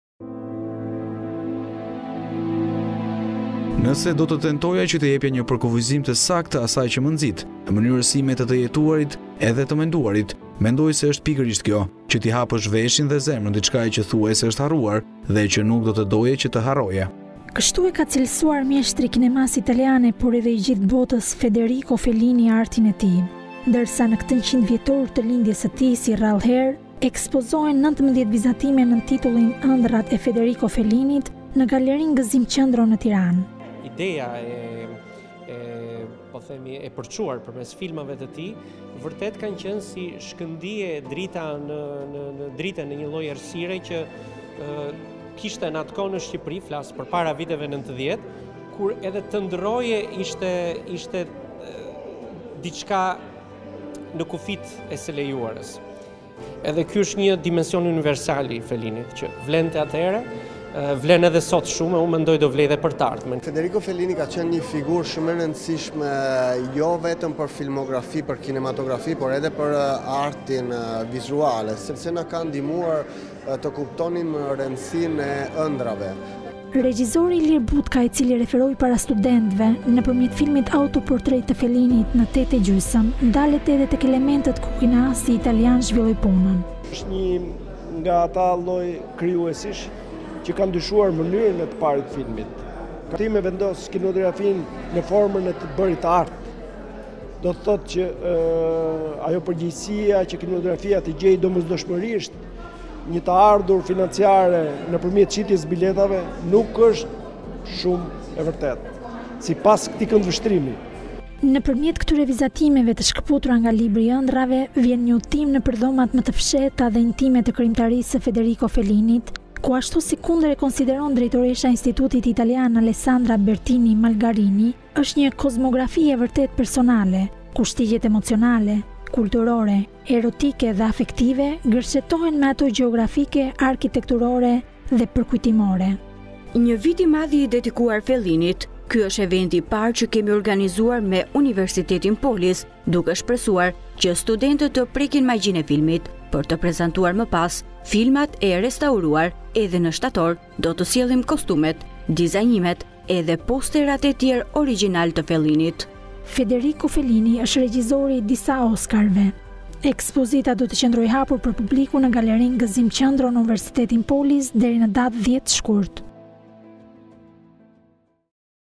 100-vjetori i lindjes, ekspozita ‘Ëndrra e Federico Fellini-t’ çelet në Tiranë
“Nëse do të tentoja që të jepja një përkufizim të saktë të asaj që më nxit, e mënyrës sime të të jetuarit edhe të menduarit, mendoj se është pikërisht kjo, që ti hapësh veshin dhe zemrën diçkaje që thuajse është harruar dhe që nuk do të doje që të harrohej”. Kështu e ka cilësuar mjeshtri i kinemasë italiane, por edhe i gjithë botës, Federico Fellini artin e tij. Ndërsa, në këtë 100-vjetor të tij të lindjes, si rrallëherë ekspozohen 19 vizatime nën titullin  “Ëndrrat e Federico Fellini-t” në galerinë “Gëzim Qëndro” në Tiranë. Pedagogu i “Dizajn urbanit” Sotir Dhamo dhe ai i “Arteve vizuale” Stefano Romano flasin jo vetëm për rëndësinë e vizatimeve, por edhe të gjurmëve të pashlyeshme të Fellinit tek artdashësit.

“Fellini përcjell një dimension të ndryshëm të kinemasë, mbi të gjitha atë të meditimit, që  nuk kuptohet me shikimin e parë, por kërkon përqendrim. Ideja e përçuar përmes filmave të tij, ka qenë si shkëndijë edhe dritë në një lloj errësire, në një moment kur në Shqipëri edhe të ëndërroje ishte diçka në kufijtë e së lejuarës. Në këtë aspekt, Fellini na bënte të kuptonim, se ëndrrat janë ajo pjesë që shtyn realitetin përpara. Ky dimension universal i Fellini-t na shtynte të ëndërronim atëherë, tani edhe do të na shtyjë edhe në të ardhmen”, shprehet pedagogu Sotir Dhamo.

“Fellini ka qenë figurë e rëndësishme jo vetëm në kinematografi, por edhe në artin vizual. Fellini edhe në filmat e shkurtër që janë realizuar ka qenë figurë e rëndësishme, sepse na ka ndihmuar që të kuptojmë rëndësinë e ëndrrave. Ne kemi nevojë që të mësojmë sërisht, sesi të ëndërrojmë”.

Regjisori  Ilir Butka, i cili shpjegoi para studentëve nëpërmjet filmit autoportret të Fellinit,  “Në tetë e gjysmë” kineastin italian, ndalet tek elementët ku mjeshtri zhvilloi punën e tij.

 “Është nga ata lloj krijuesish, që ka ndryshuar mënyrën e të parit të filmit. Është interesante të shikojmë sesa aktuale ishte ajo që ai tentoi, që të sjellë një kinematografi të shprehjes artistike. Nëse është kinematografia art apo industri, ai padiskutim e vendos këndvështrimin e tij në mënyrën e të bërit art, si shprehje artistike dhe jo si industri”, shpjegon Butka.